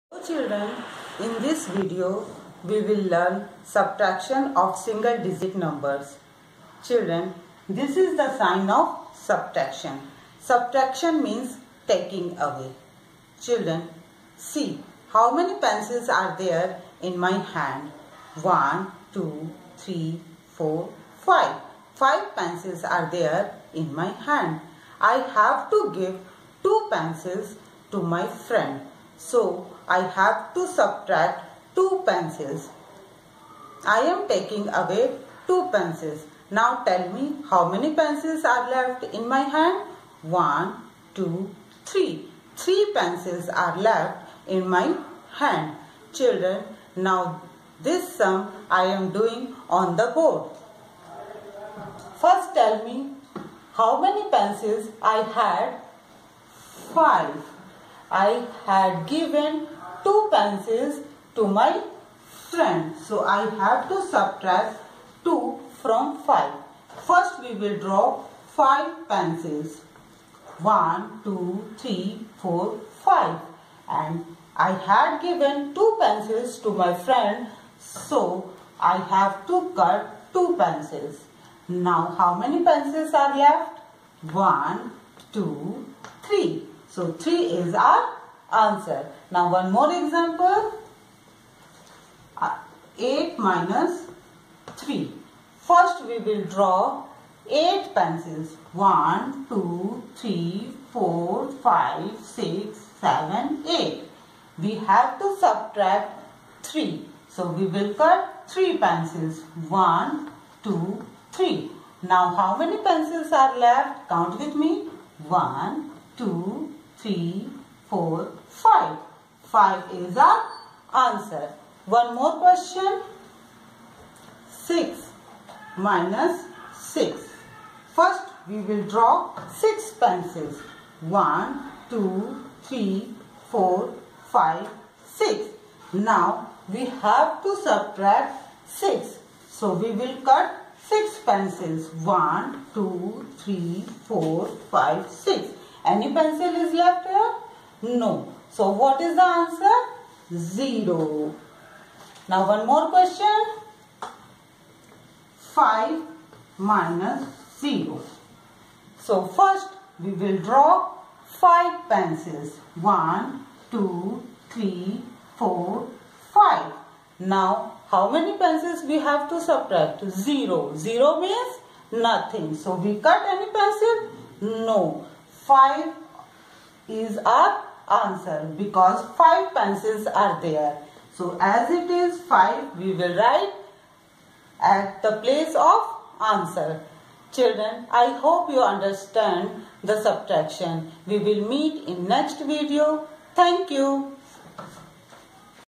Basic concept of Subtraction || Easy way of Subtraction || Planet Maths
Hello kids,
In this video you will learn how to Subtract two single digit numbers in a fun and easy way.